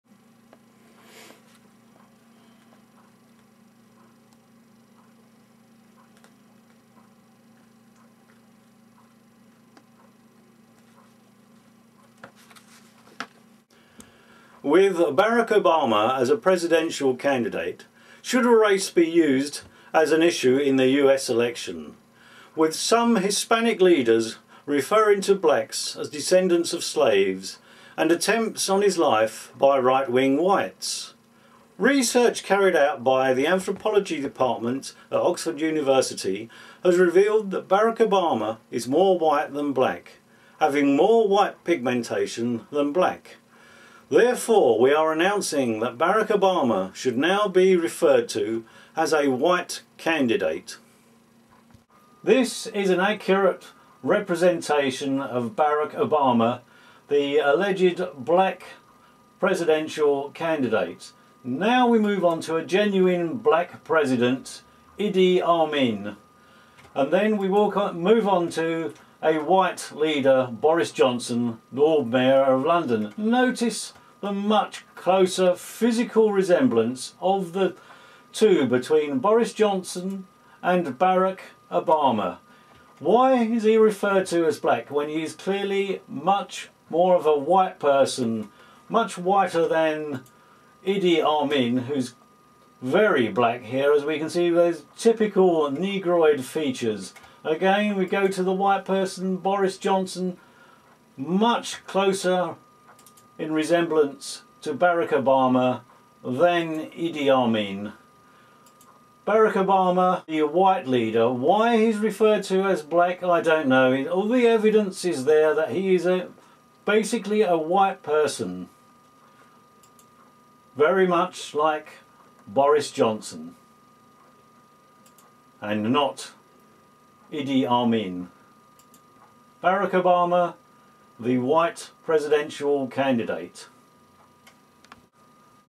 BARRACK OBAMA IS WHITE and BLACK!
Sign this petition thanks! Equal Rights for England and Every Citizen.
WITH RACE PLAYING A BIG PART IN THE US PRESIDENTIAL RACE, HIPANIC LEADERS CLAIMING THEY ARE DESCENDANTS OF THE VICTORIOUS SPANISH INVADERS AND THAT BLACKS ARE DESCENDANTS OF SLAVES, SOME WHITES PLOTTING TO KILL OBAMA, RESEARCH CARRIED OUT BY THE ANTHROPOLGY INSTITUTE HAS REVEALED THAT OBAMA HAS MORE WHITE PIGMENTATION THAN BLACK-SEE FOR YOURSELVES...